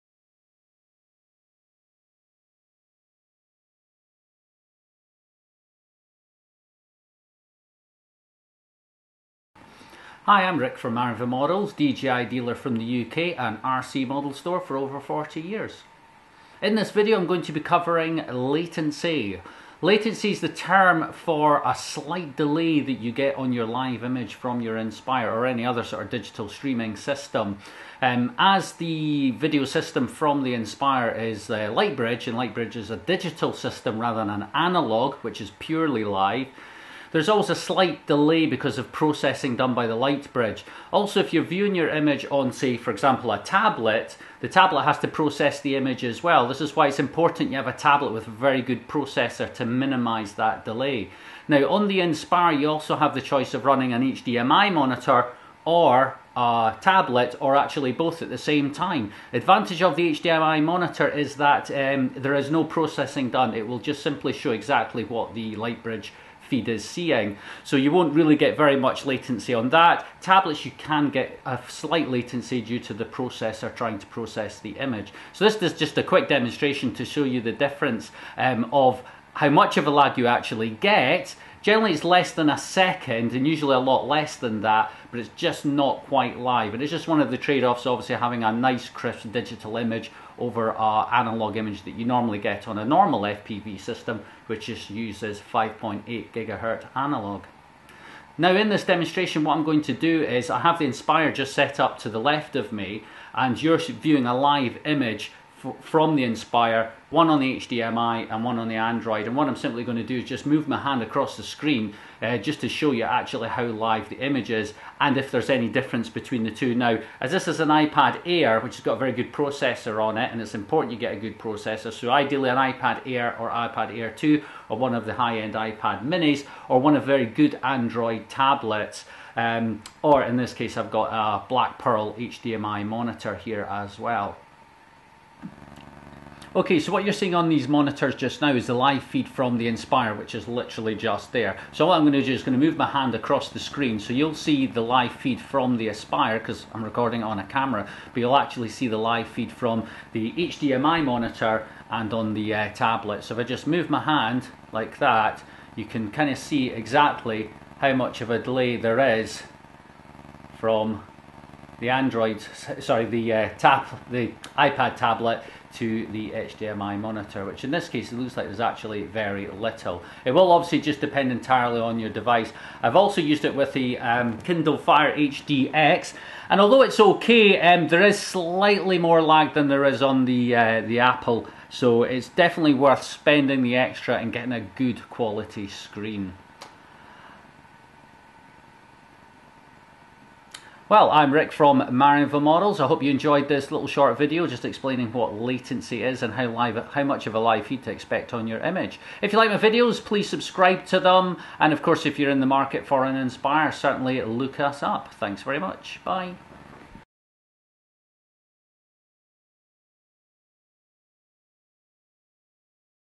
DJI Inspire 1 Lightbridge, Video Latency Explained with IPad Air & HDMI Screen
Latency is the slight delay or lag you get when streaming a digital video signal. The is video shows the difference from a live feed to a digitally processed feed from the built in Light bridge system of the DJI Inspire.